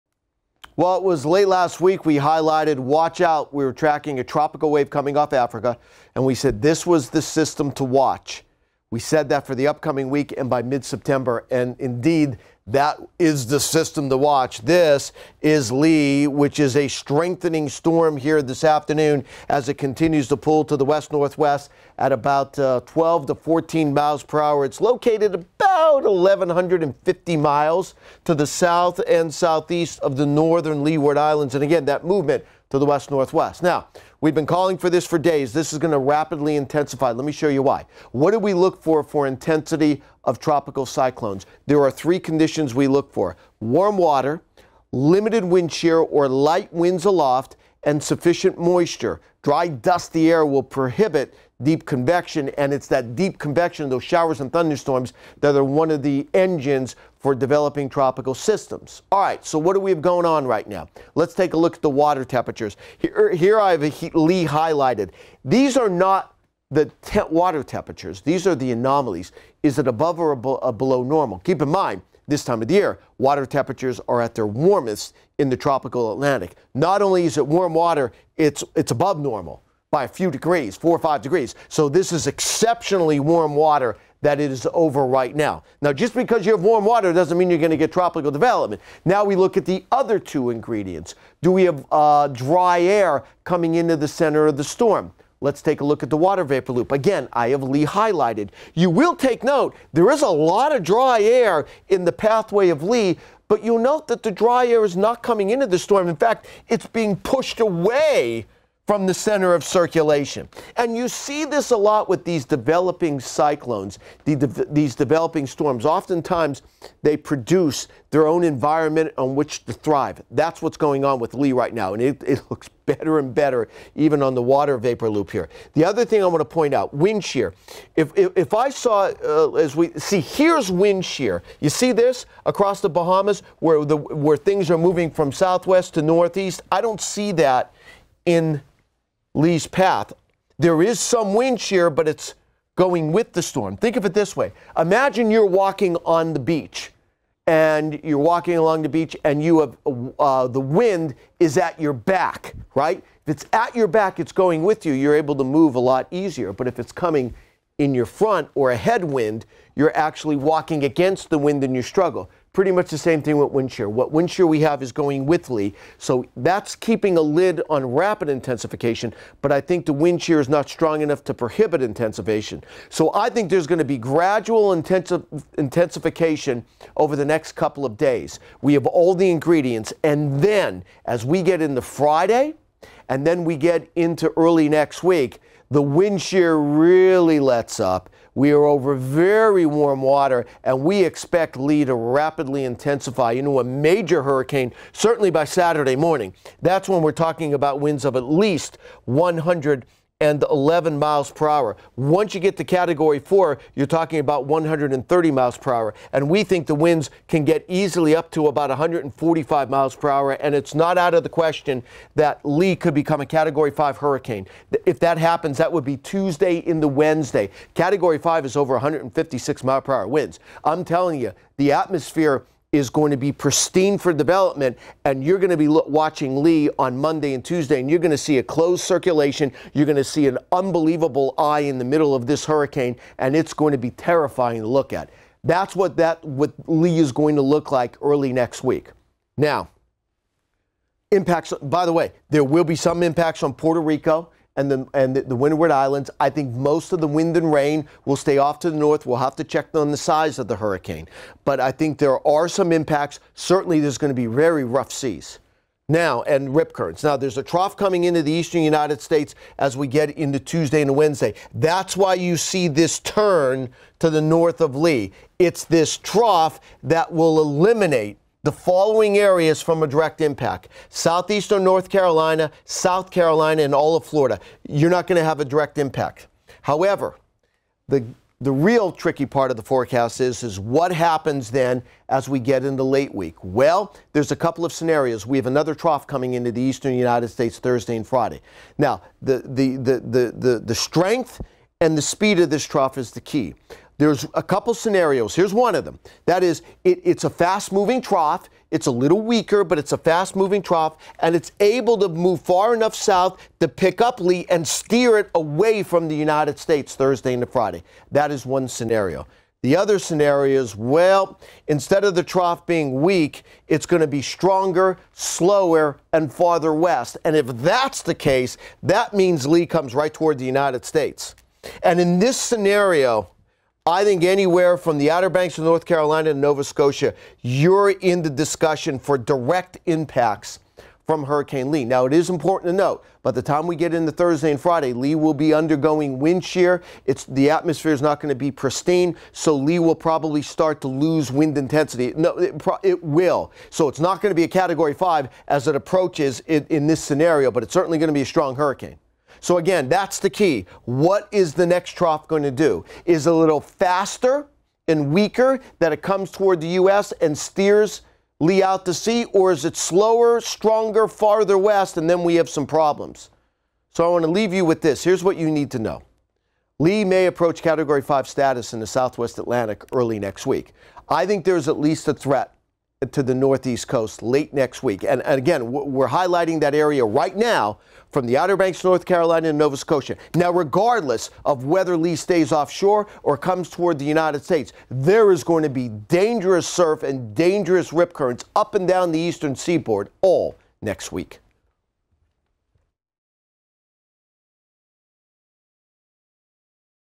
Hurricane Lee UPDATE: Cat 5 but Two Possible Scenarios | AccuWeather
Lee will become a hurricane at 5 PM EDT Wednesday and will continue to rapidly intensify, turning into a Category 5 hurricane over the weekend. Will it affect the U.S. East Coast? AccuWeather Chief Video Meteorologist Bernie Rayno says there are two scenarios.